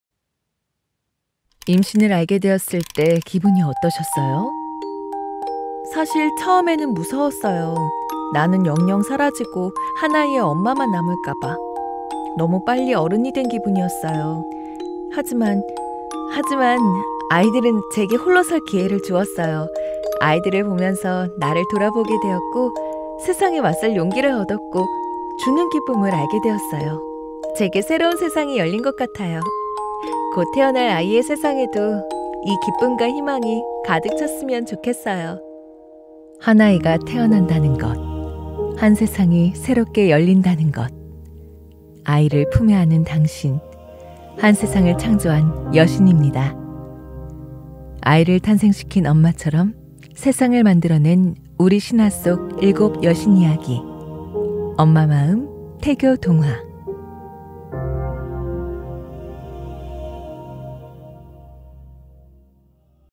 Q. 임신을 알게 되었을 때, 기분이 어땠나요?┃엄마 마음, 태교 동화
Q. 임신을 알게 되었을 때, 어떤 기분이었나요?
예비맘의 진솔한 이야기를 들어보세요.

우리 아이 홈스쿨링, 함께 공부해 보세요!

엄마에게 도움되는 정보, 아래에서 확인하세요!